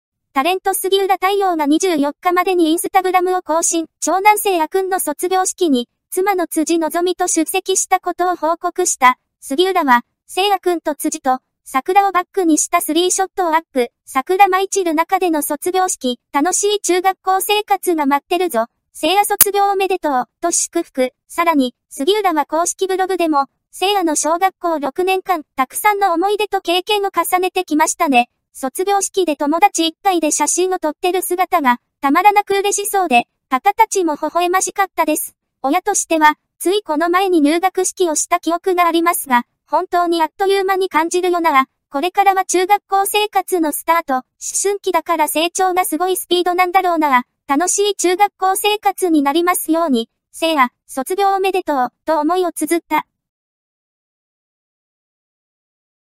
杉浦太陽が長男卒業式に辻希美と出席！！
長男青空（せいあ）くんの卒業式に、妻の辻希美（35）と出席したことを報告した。